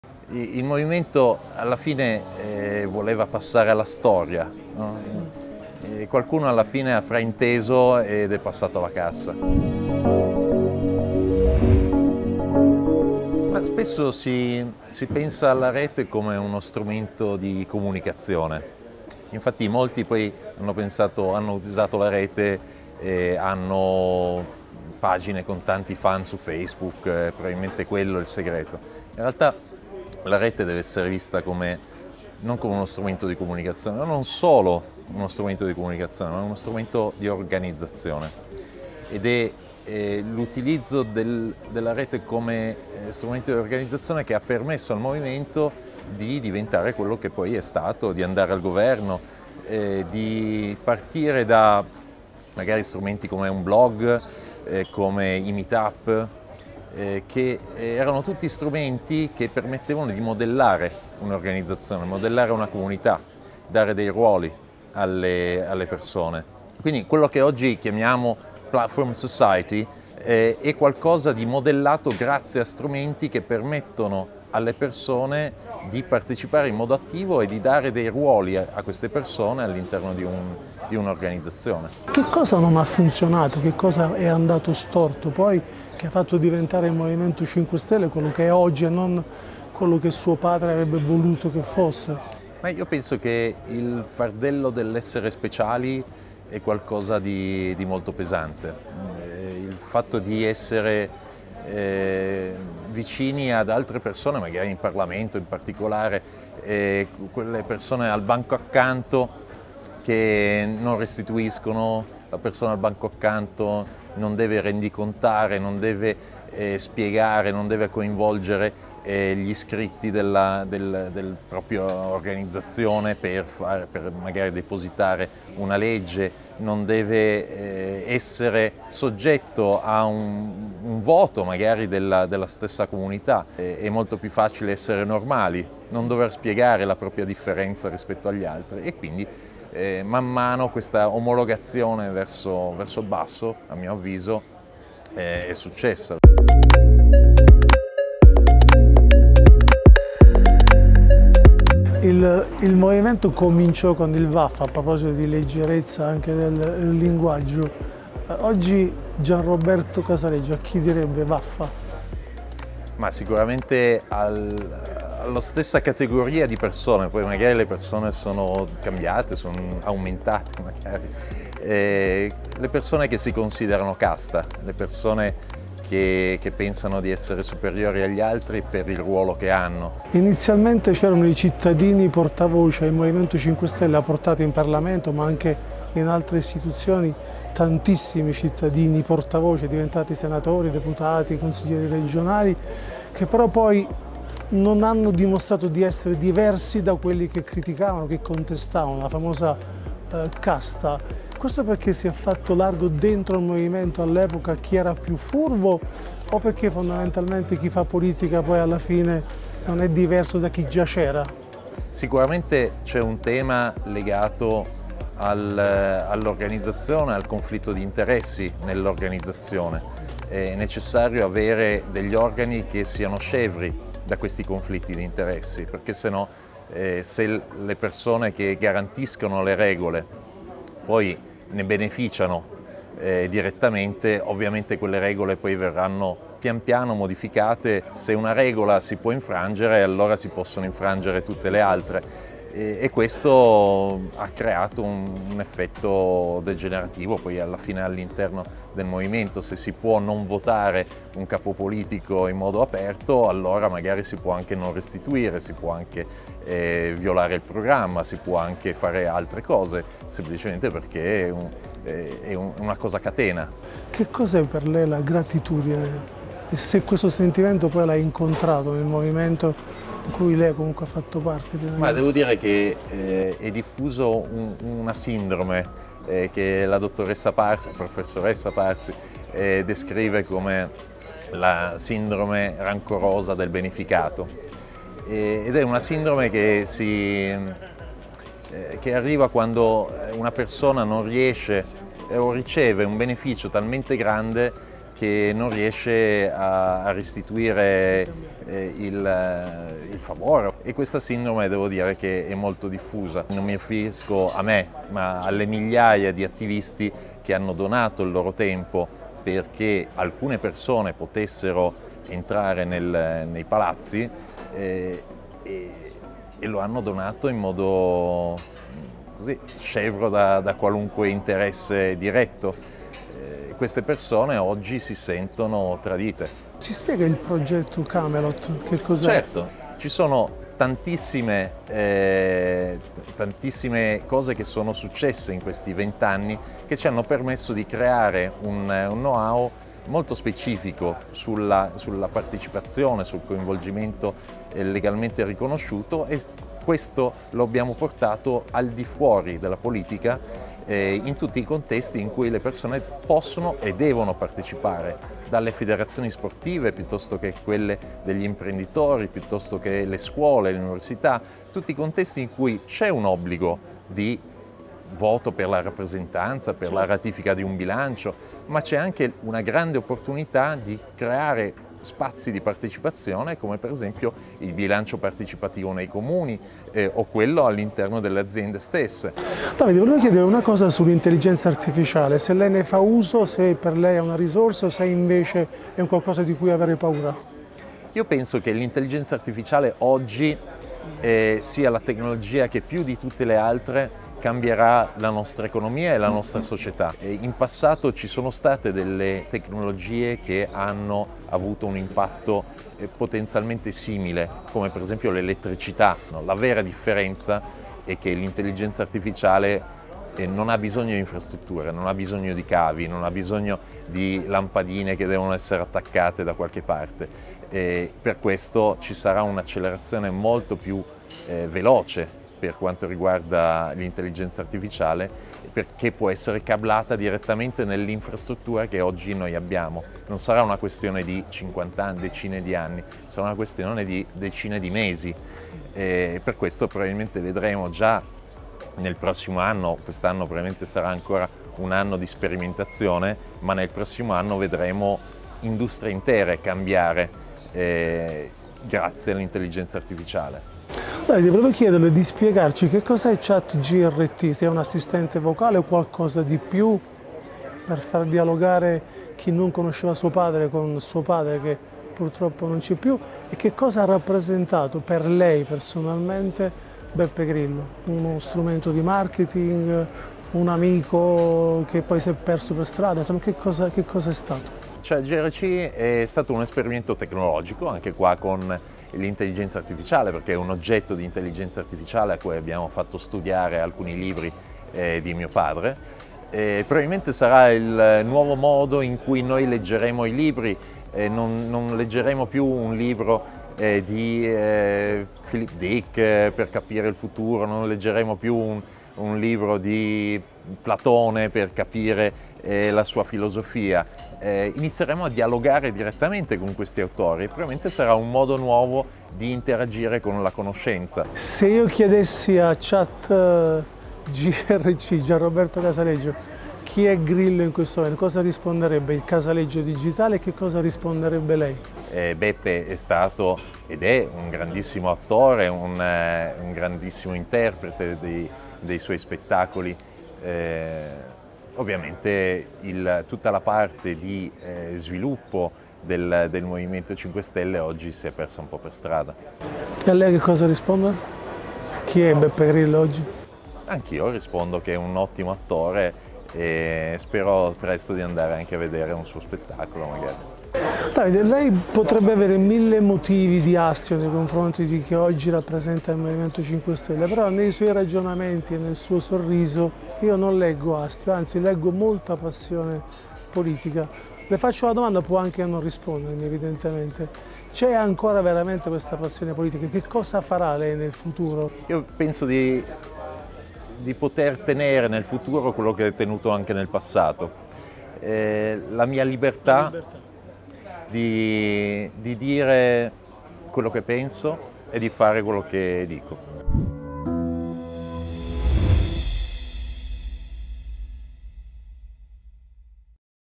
Intervista a Davide Casaleggio: M5S, la differenza tra passare alla storia e passare alla cassa
Davide Casaleggio parla a Fortune Italia di AI, piattaforma Rousseau, progetto Camelot e di come Conte in 15 mesi ha distrutto il progetto politico del M5S ideato dal padre e da Beppe Grillo.

Che cosa c’è nel suo futuro? La libertà “di dire quello che penso e di fare quello che dico”.

Intervista di Paolo Chiariello